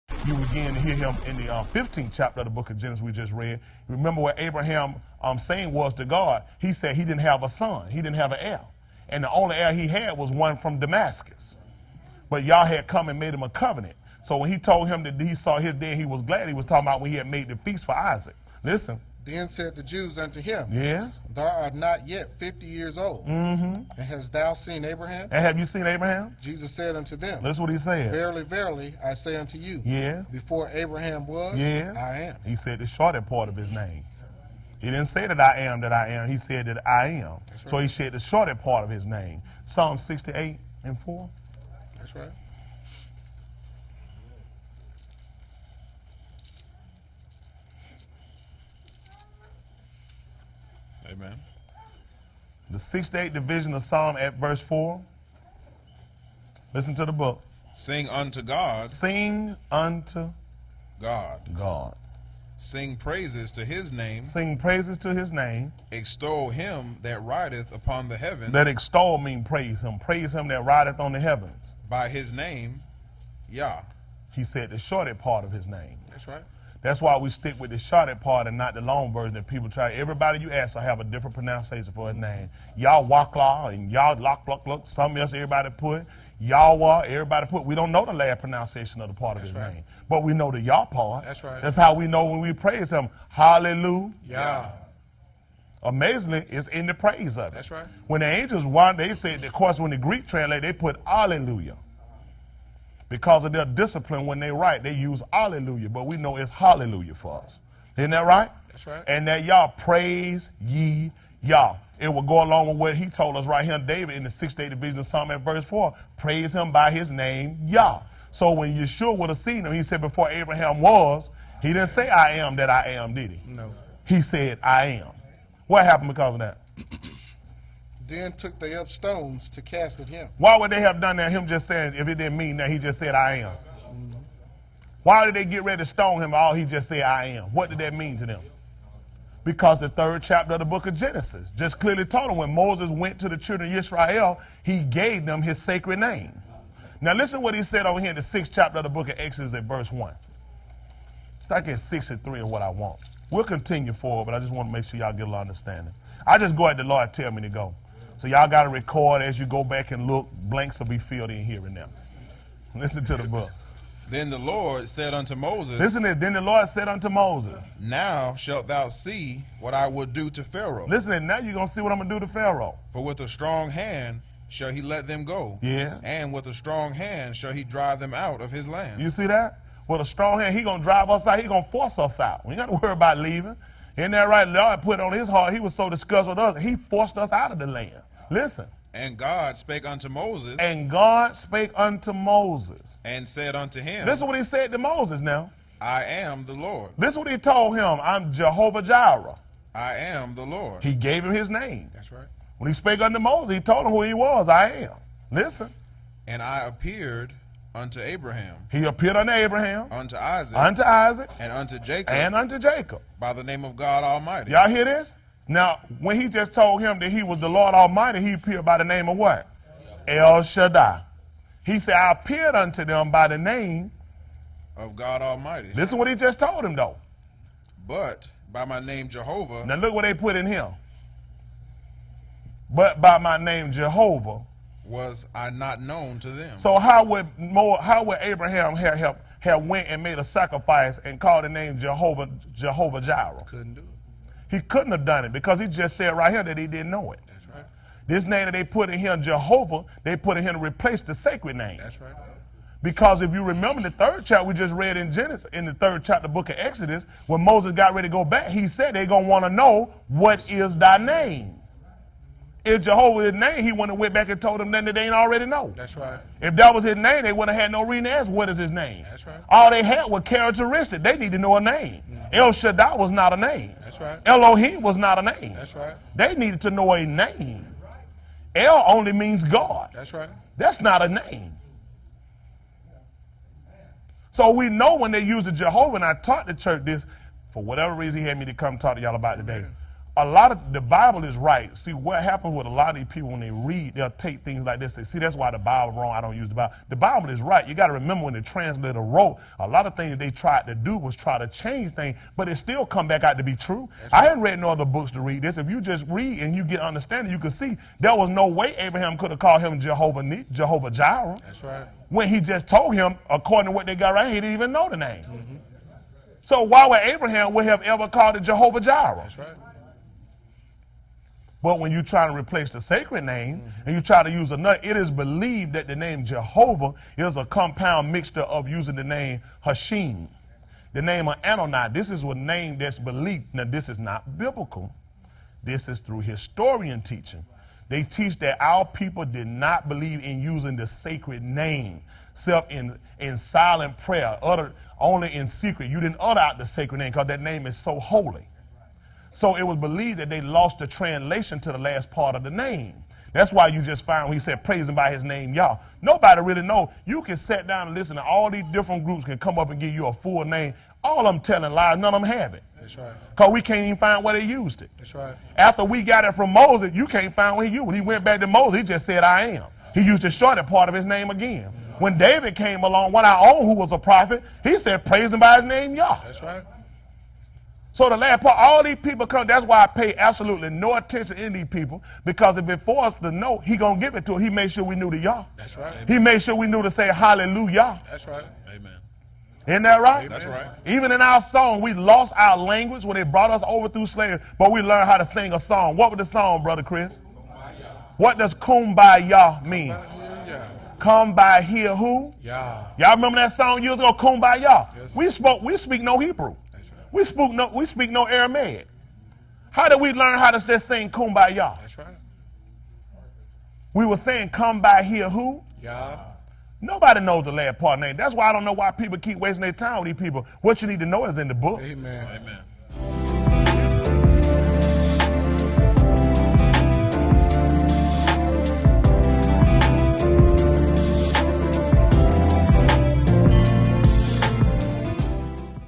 Jehovah is NOT God's Name! (Pt. 2)
Pastor Smith expounding once again on how the name "Jehovah" is not the Most High's name.